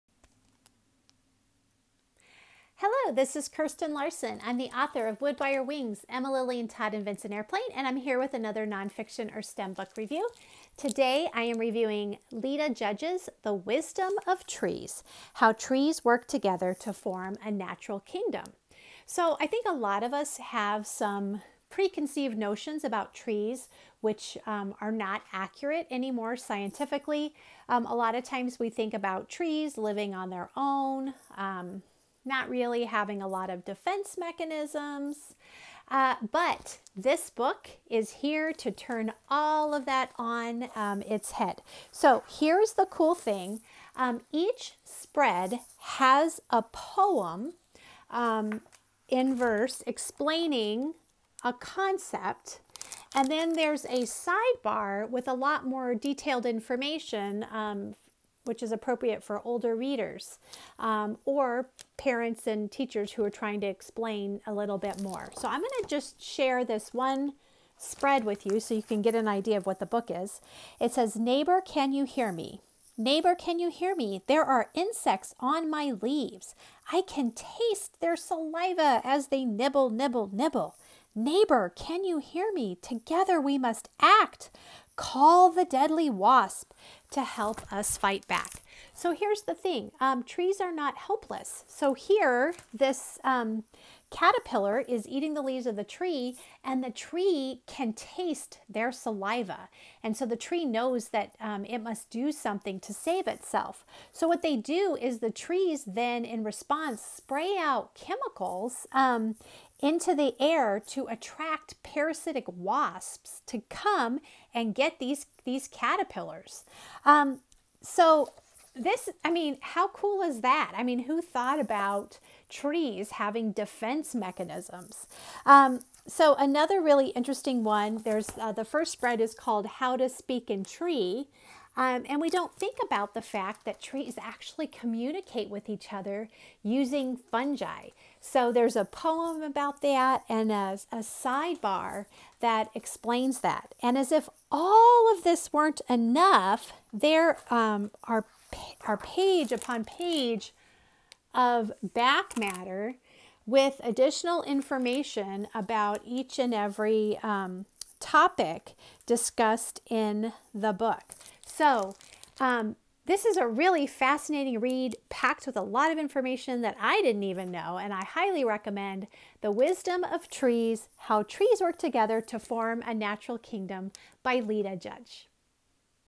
NF/STEM Book Reviews: The Wisdom of the Trees
It's spring! The perfect time to read Lita Judge's THE WISDOM OF TREES: How Trees Work Together to Form a Natural Kingdom. This is the type of STEM book I love -- packed with new information and discoveries I'd never known before. Judge's main text is lyrical and simple, yet each spread has detailed sidebars for older readers. A great companion to CAN YOU HEAR THE TREES TALKING.